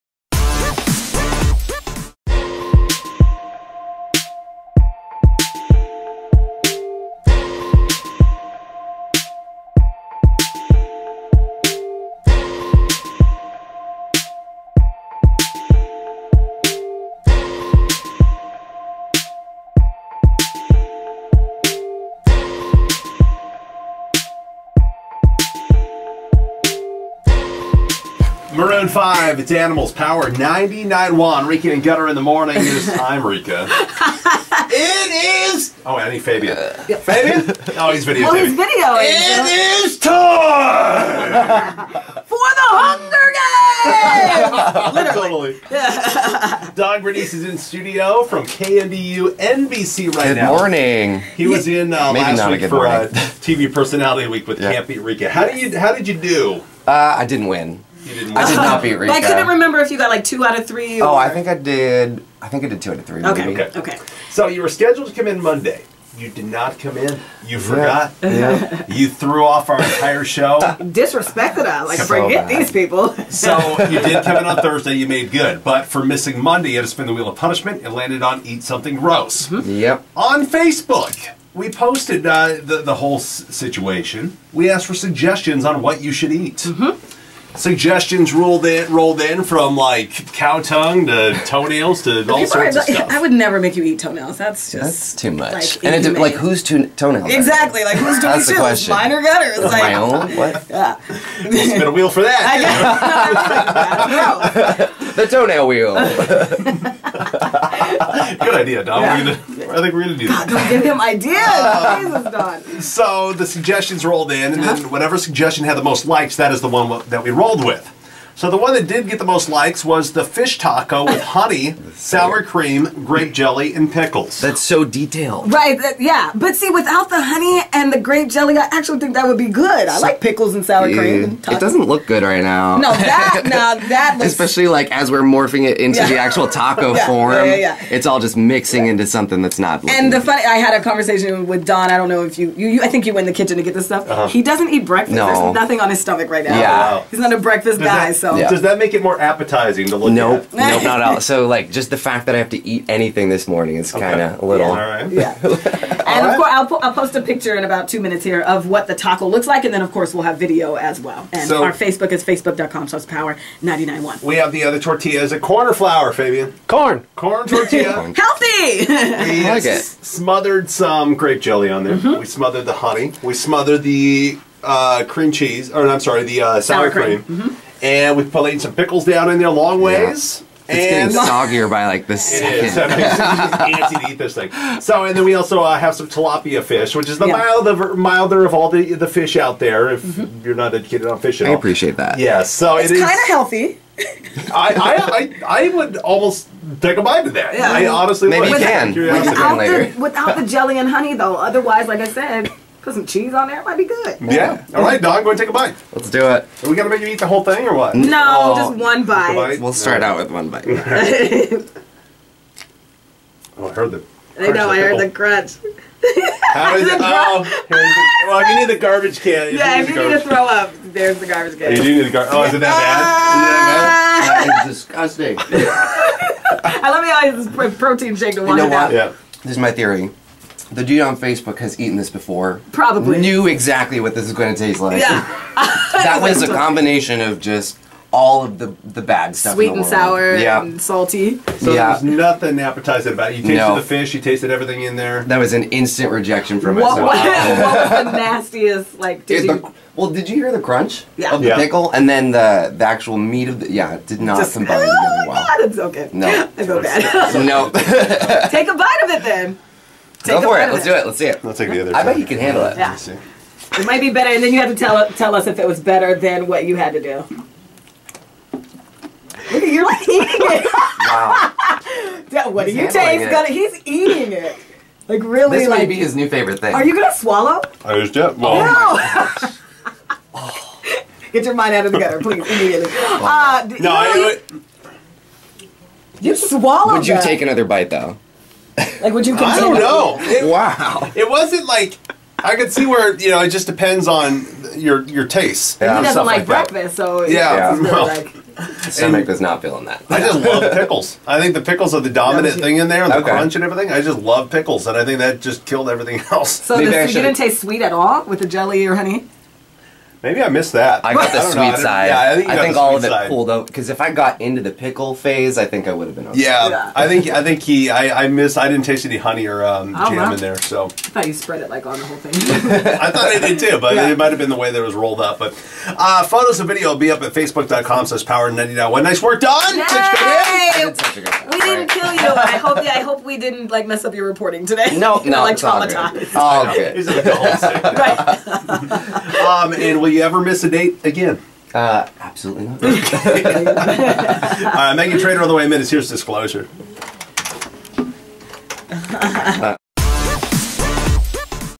R&G: Eat Something Gross 11/25/14
Don Granese from KNDU-TV was in studio to pay off on his punishment to "Eat Something Gross". (Don forgot to show up last Monday for "Can't Beat Reka" TV Personality Week). Listeners suggested the "Gross" thing Don was to eat, and the most votes went to the Fish Taco with Sour Cream, Grape Jelly, Honey and Pickles.  Check out the in-studio video!